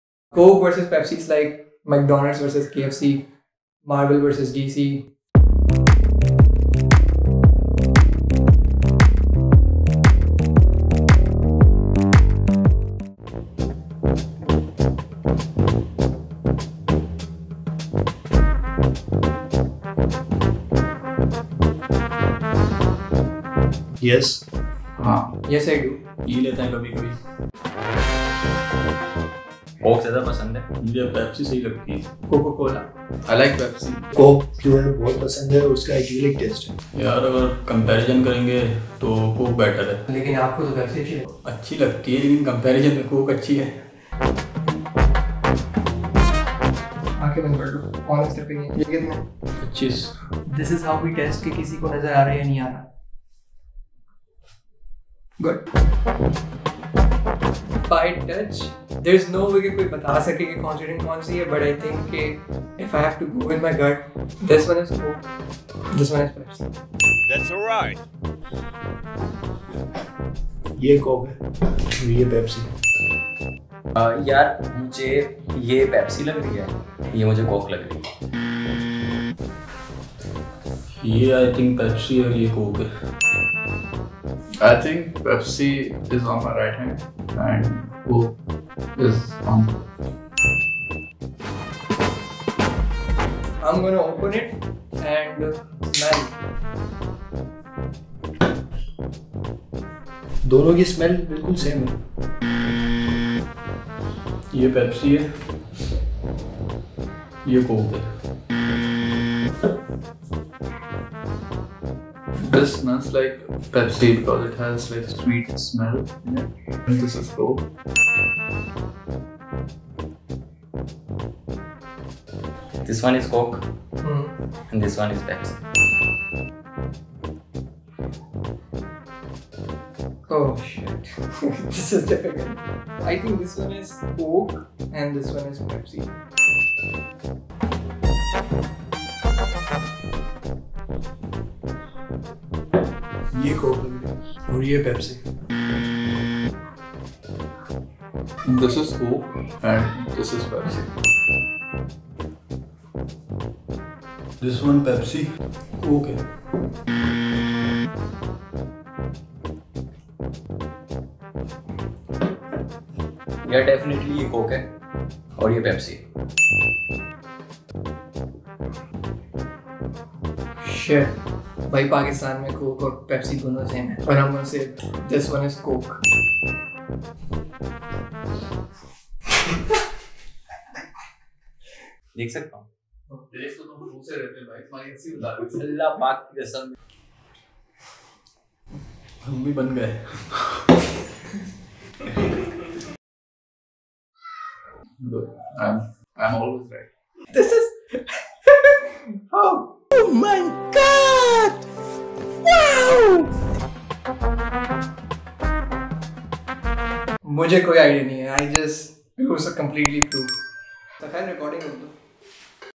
Can normal people tell difference between Coke and Pepsi Blindfolded
We invited our followers and did blindfold test with them to check if they can tell difference between Coca Cola and Pepsi.

Like and Follow to stay updated. DM us if you want to be part of our videos.